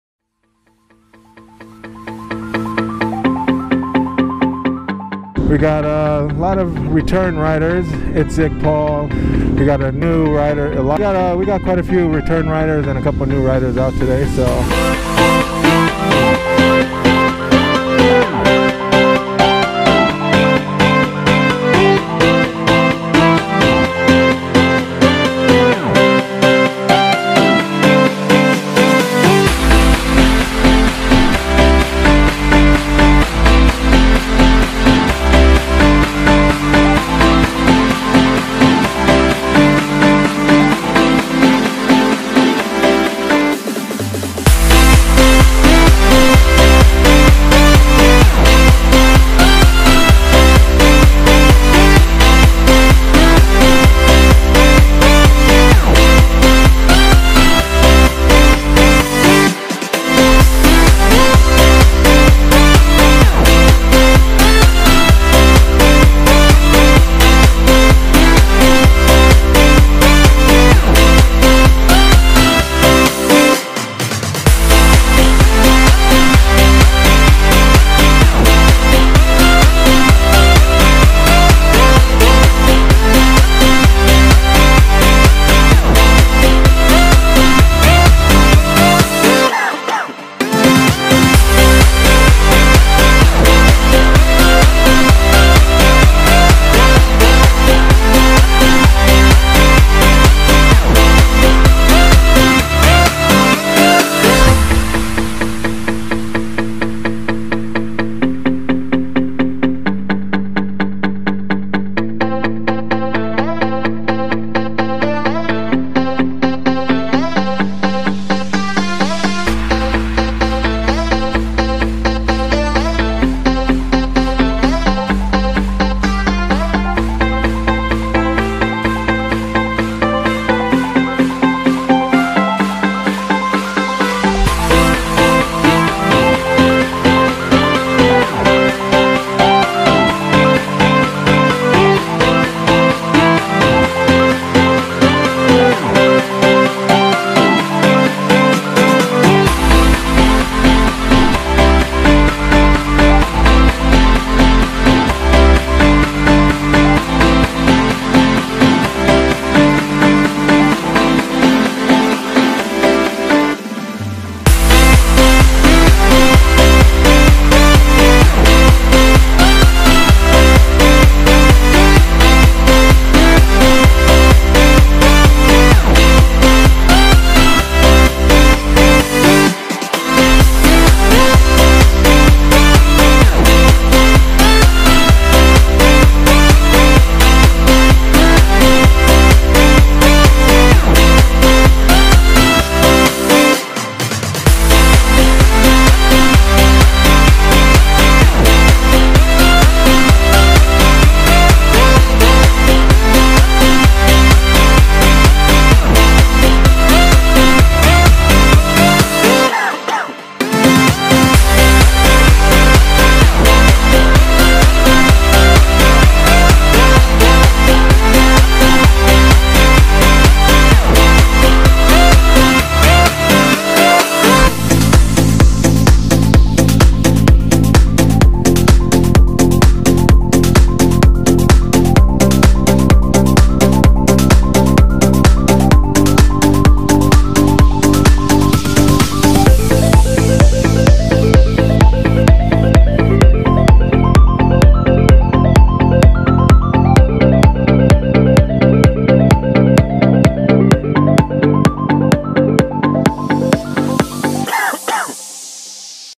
NCR| Saturday Morning Br8fast Ryde| Rainbow Oak Restaurant| Sweet NSX Bruh| 2-15-20| Edited
Highlights of NCR Saturday Morning Br8fast Ryde out to Rainbow Oak Restaurant. Awesome Saturday Morning Br8fast Ryde to Rainbow. Amazing Group, Seen the new Nissan NSX on highway 76.  Try to catch it, but it was way to fast lol 😂😂